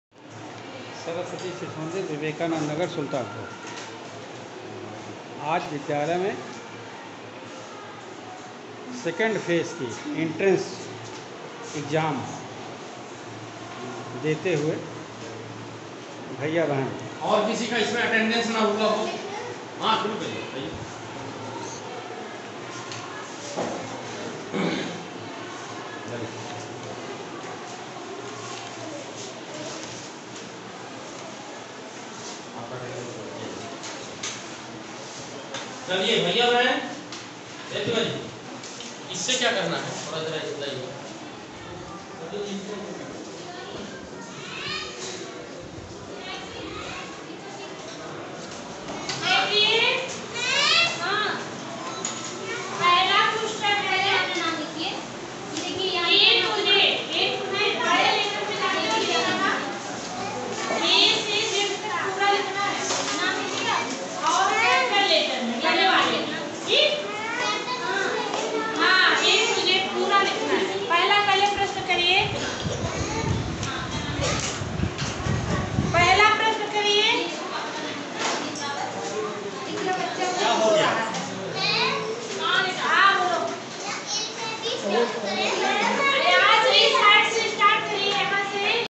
सुलतानपुर: सरस्वती शिशु मंदिर विवेकानंद नगर में द्वितीय चरण की प्रवेश परीक्षा देते भैया बहन-2
सुलतानपुर: सरस्वती शिशु मंदिर विवेकानंद नगर में द्वितीय चरण की प्रवेश परीक्षा देते भैया बहन